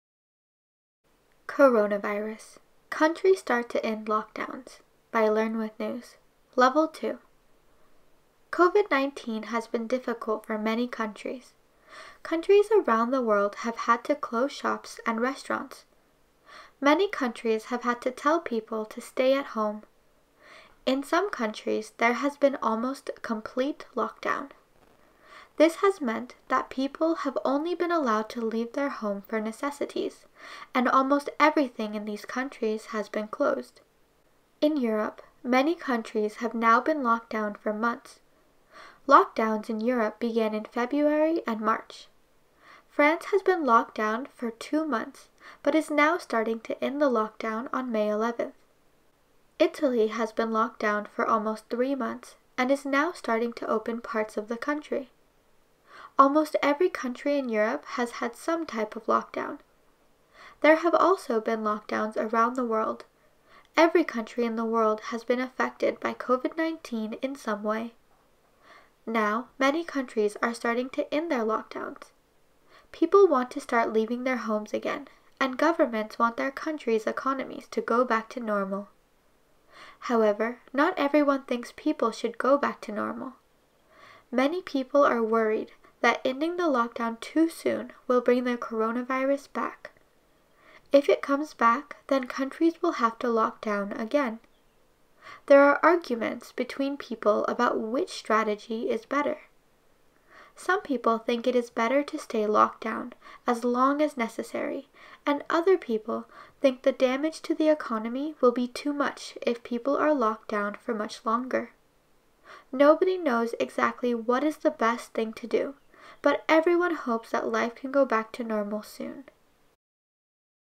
Coronavirus: Countries Start to End Lockdowns - Level 2 - American English
This is a level 2 news story from Learn With News. For more English learning activities on this story